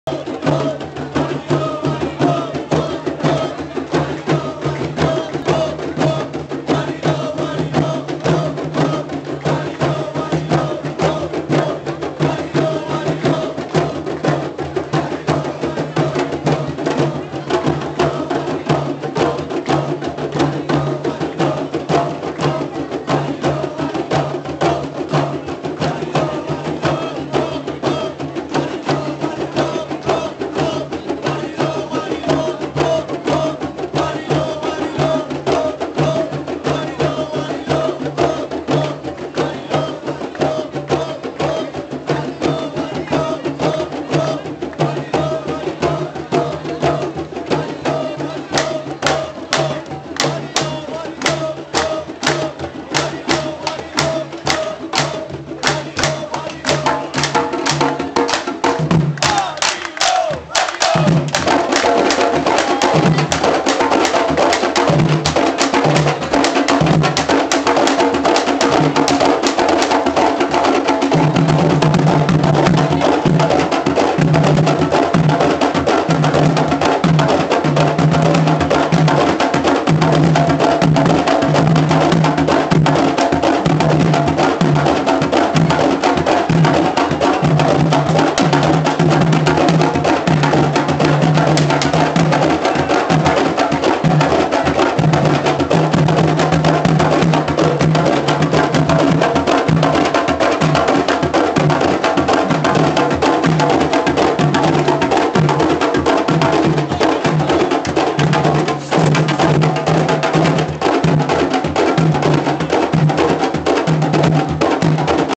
guarilo llamada 2008
!!Guarilo!!!comparsa de candombe representantes de lomas de zamora. llamada de tambores argentina 2008